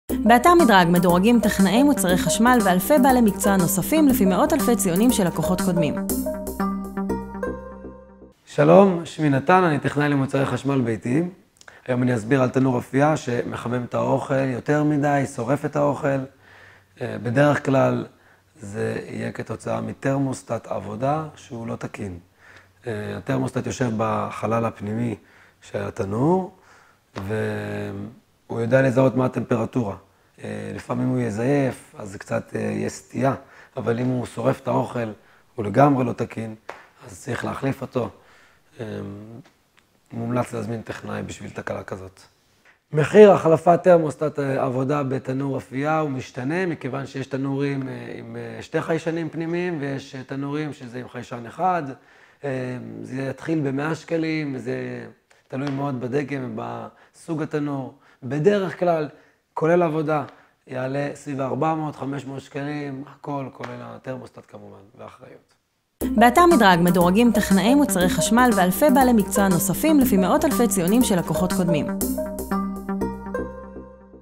תנור מחמם יותר מדי - מה עושים?
תנור שמחמם יותר מדי שורף את האוכל וגורם לנו הרבה עוגמת נפש. טכנאי תנורים מסביר איך תרמוסטט תקול יכול לגרום לתנור לחמם יותר מדי ומה המחיר של החלפת תרמוסטט. לטכנאי תנורים מומלצים לטיפים נוספים על תנורים